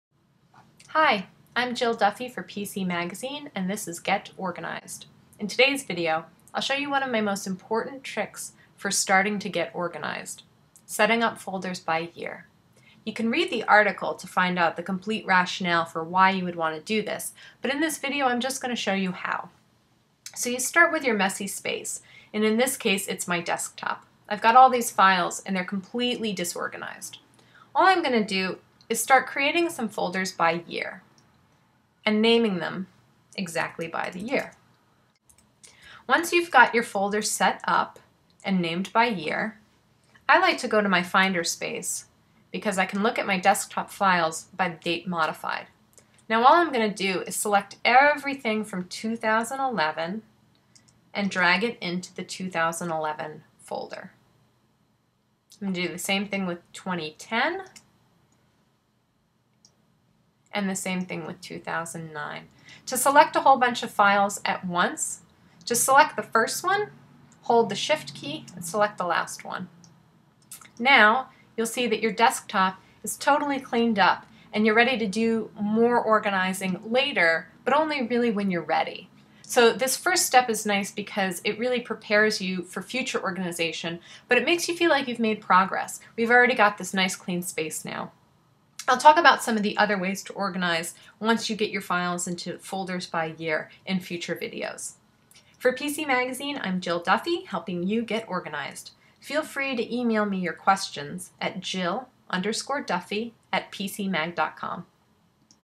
Get Organized: Using Folders by Year
Setting up folders by year is one of the first steps to organizing your computer files. See also Using Folders to Unclutter Your Desktop